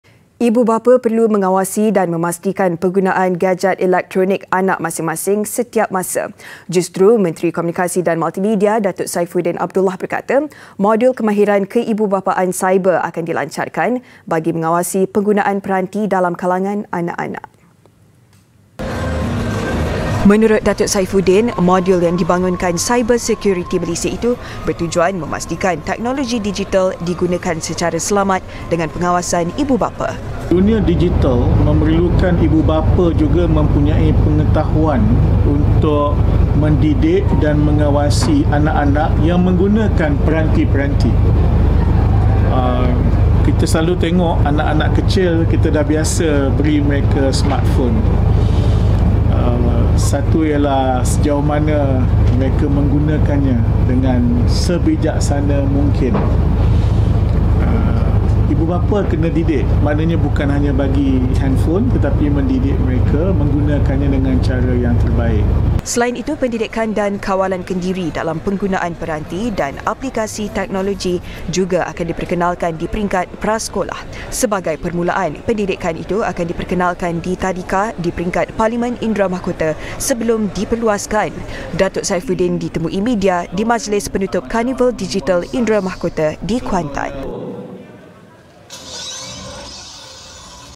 TEKNOLOGI DIGITAL | Perkenal Modul Ibu Bapa Awasi Pengunaan Gajet
Ibu bapa perlu mengawasi dan memastikan penggunaan gajet elektronik anak pada setiap masa.